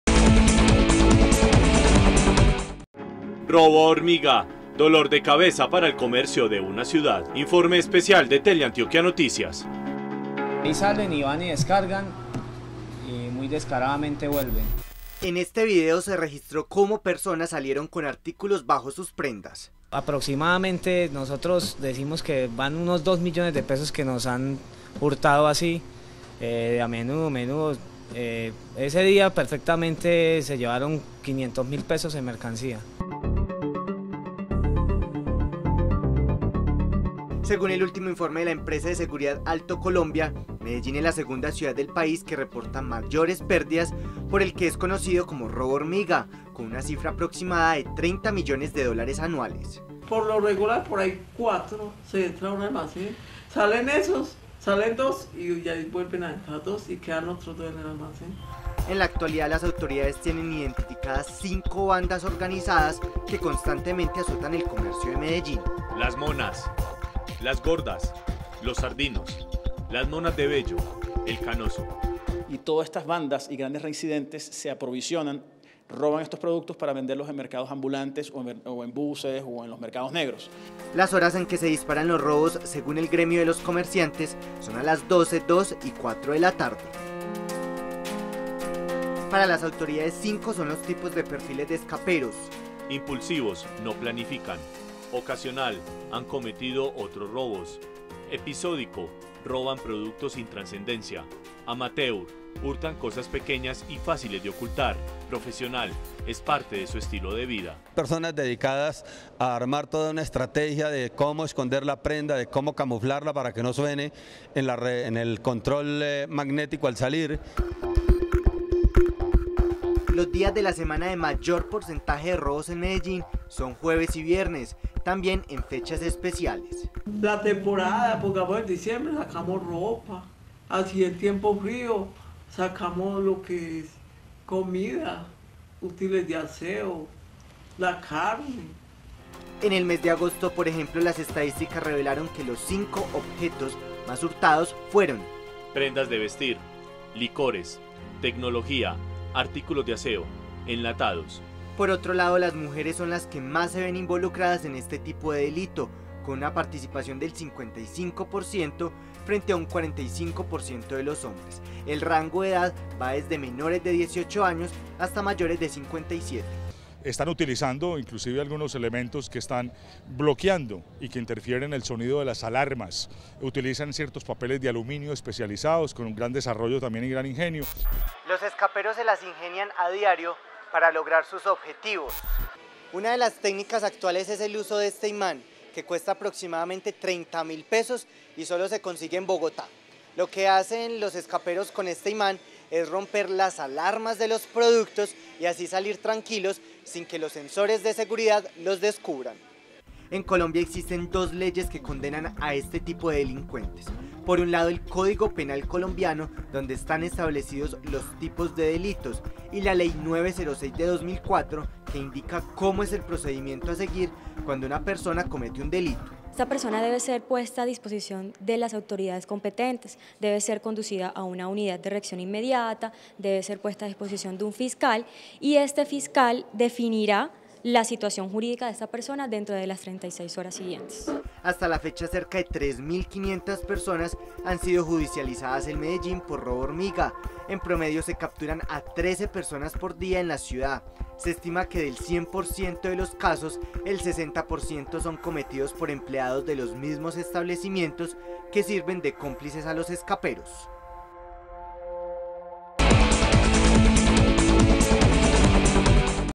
“Robo Hormiga”, dolor de cabeza para los comerciantes de la ciudad
Teleantioquia Noticias conoció cómo se mueve en la ciudad el llamado “Robo Hormiga”, una modalidad de hurto que a diario deja millonarias pérdidas al comercio de Medellín.

¿Cuáles fueron los productos más hurtados ? ¿Cómo se judicializan a las personas que se dedican a este tipo de hurto? ¿Y cuáles son las bandas que en la actualidad azotan el comercio de la ciudad? Los detalles a continuación en “Robo hormiga”, dolor de cabeza para el comercio de una ciudad, Informe Especial, Teleantioquia Noticias.